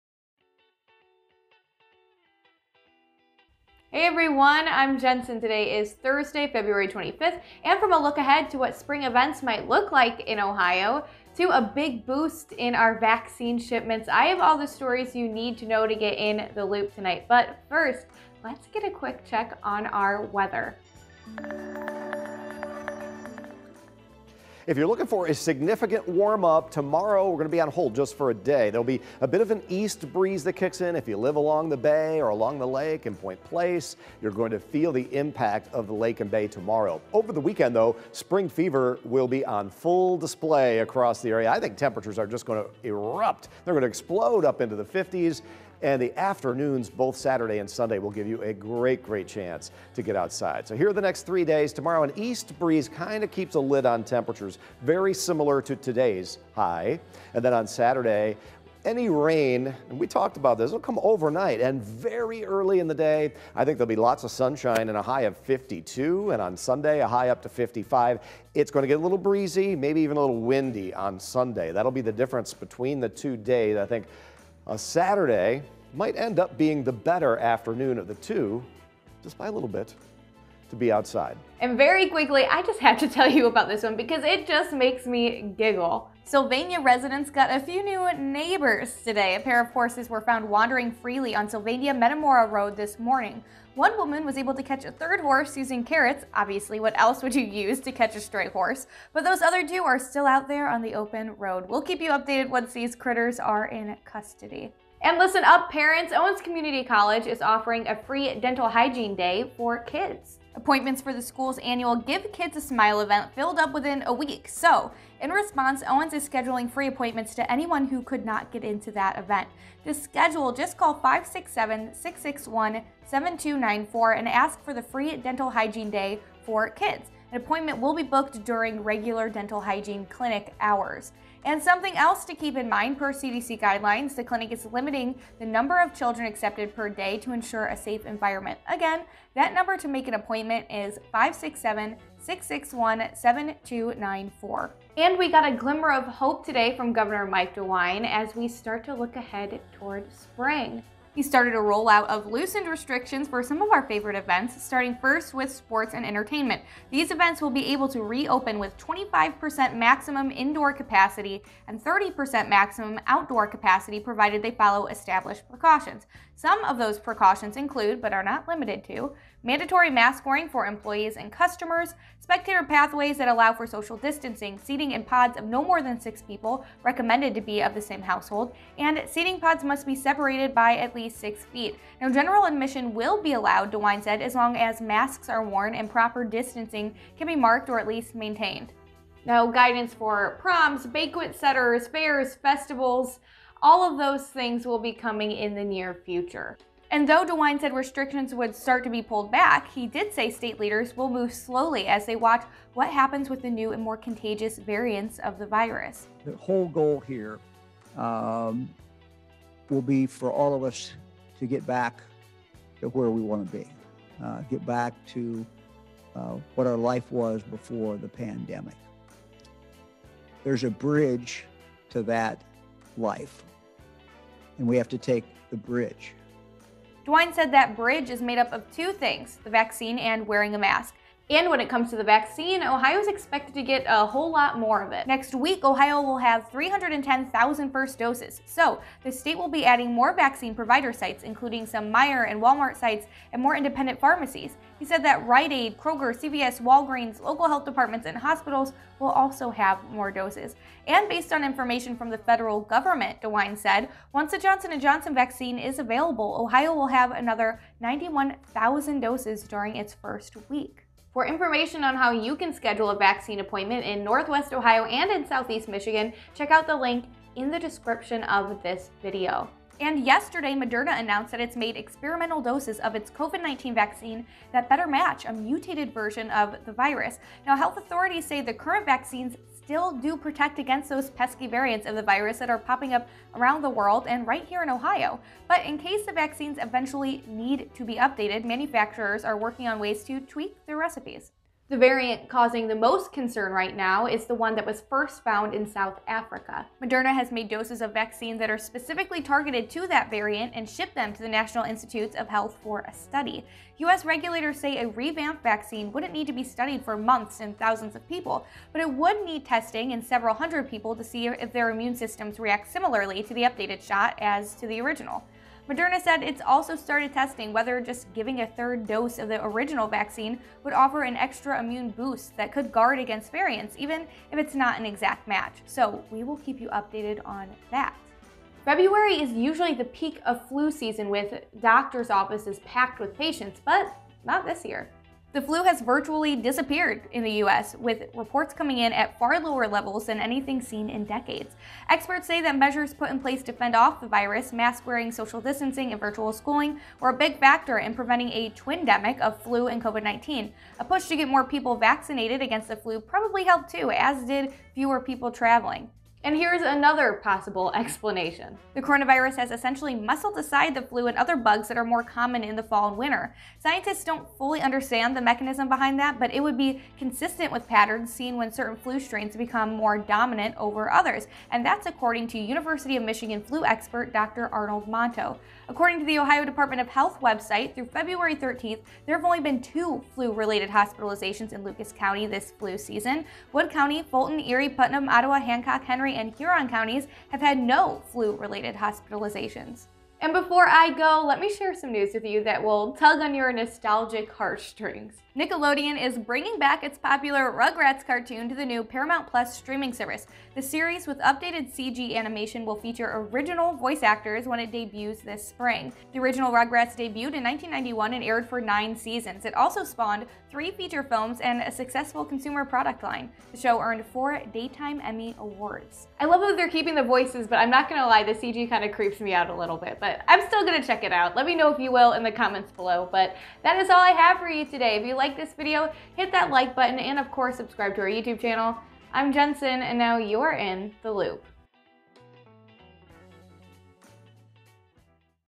Restrictions loosen in Ohio for spring events, Moderna combats COVID-19 variants | In the Loop
Here are the top headlines from Feb. 25, 2021 to get you in the loop:

✅ Horses run loose in Sylvania
✅ Owens CC has "Free Dental Hygiene Day" for kids
✅ Sports and entertainment can return in Ohio under limited capacity
✅ Moderna makes experimental vaccine to combat COVID-19 variants
✅ '90s kids rejoice! Rugrats is coming back... but it looks a little bit different.